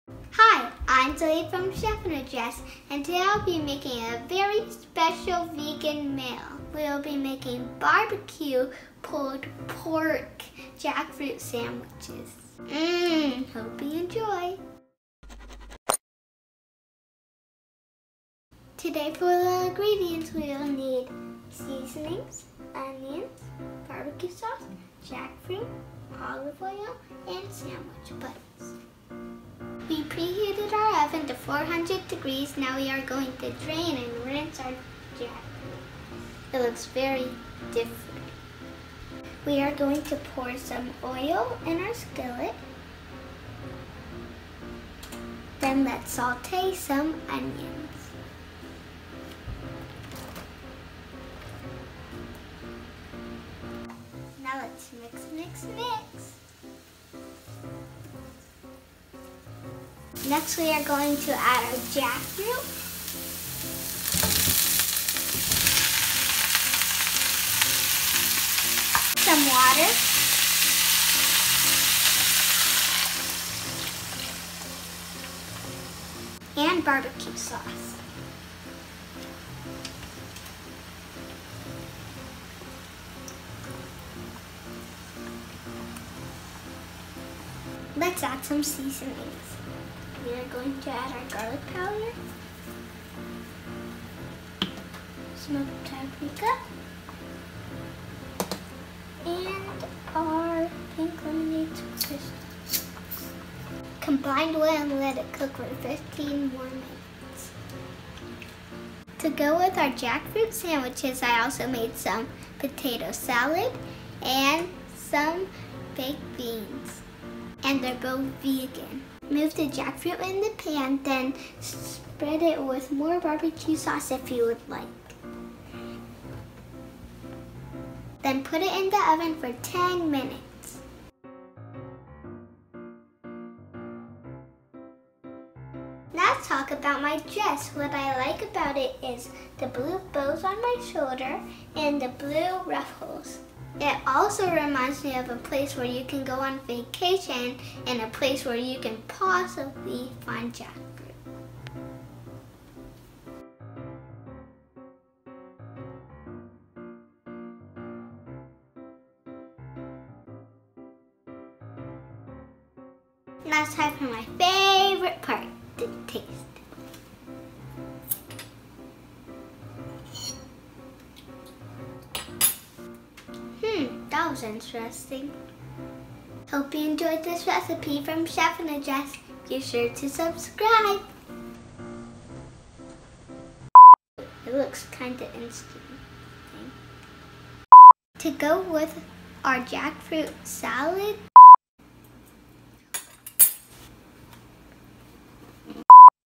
Chef In A Dress | BBQ "Pulled Pork" Jackfruit Sandwich
Today on Chef In A Dress, I’m making a vegan BBQ “Pulled Pork” Jackfruit Sandwich! This meal was very interesting… Do you like Jackfruit?

BBQ “Pulled Pork” Jackfruit Sandwich Ingredients:

- Seasonings
- 1 Onion
- 1 cup BBQ Sauce
- 2 cans Green Jackfruit
- 1 tbsp Olive Oil
- Hawaiian Buns

About Chef In A Dress
Hi, I'm Zoe! Thanks for checking out my YouTube Channel. I love hanging out with my family, talking about my favorite dresses and cooking my favorite dishes.